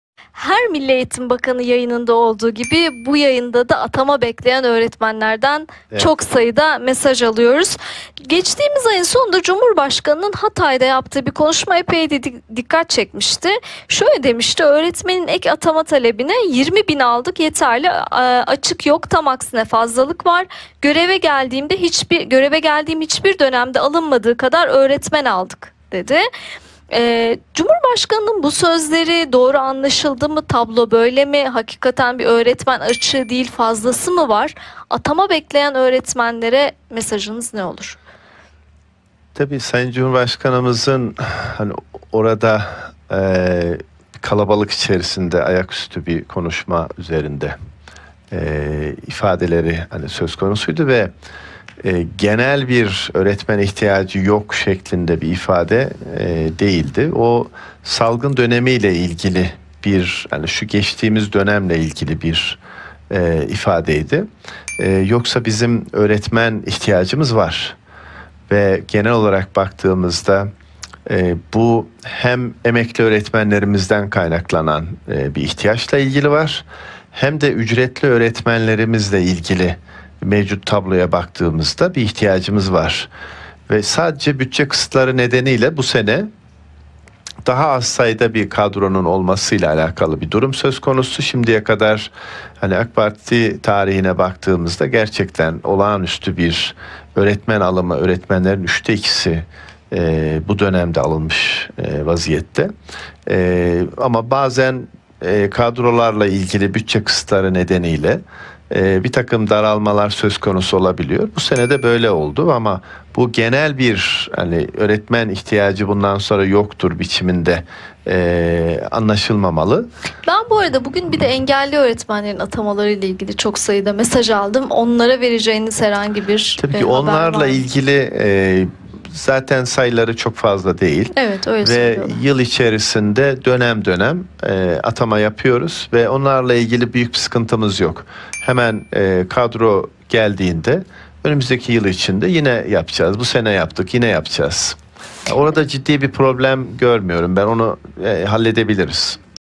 EK ATAMA OLACAK MI 2021 /2021 ÖĞRETMEN ATAMALARI /ZİYA SELÇUK EK ATAMA SORUSUNU CEVAPLADI !
Tv programına katılan Ziya Selçuk ek atama sorusuna cevap verdi.

Detaylar videoda

Takip edebilirsiniz